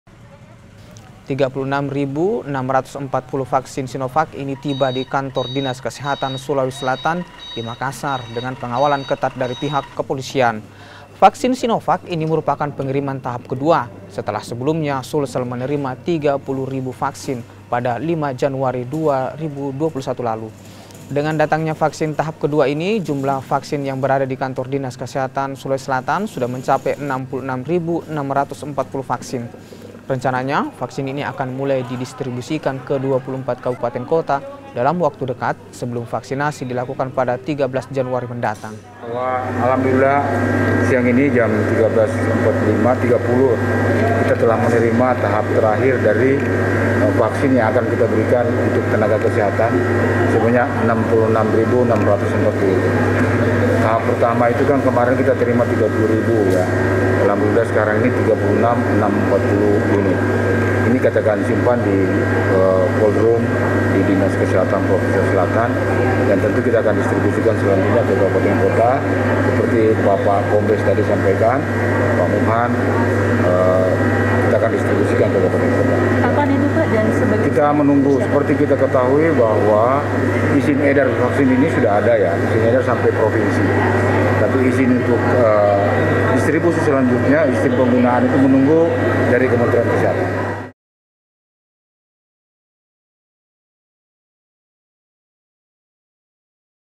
Sulawesi Selatan Kembali Terima Vaksin Sinovac Tahap Kedua
MAKASSAR, KOMPAS.TV - Sulawesi selatan kembali menerima vaksin sinovac sebanyak 36 ribu 600 vaksin . Vaksin ini merupakan vaksin tahap kedua yang diterima , setelah sebelumnya , sulsel manerima 30 ribu vaksin sinovac .

36 ribu 640 vaksin sinovac ini tiba di kantor dinas kesehatan sulawesi selatan , di makassar dengan pengawalan ketat dari pihak kepolisian .

Vaksin sinovac ini merupakan pengiriman tahap kedua , setelah sebelumnya sulsel menerima 30 ribu vaksin , pada 5 januari 2020 lalu .

Dengan datangnya vaksin tahap kedua ini , jumlah vaksin yang berada di kantor dinas kesehatan sulsel sudah mencapai 66 ribu 640 vaksin .

Rencannya , vaksin ini akan mulai didistribusikan ke 24 kabupaten,kota dalam waktu dekat , sebelum vaksinasi dilakukan pada 13 janauri mendatang .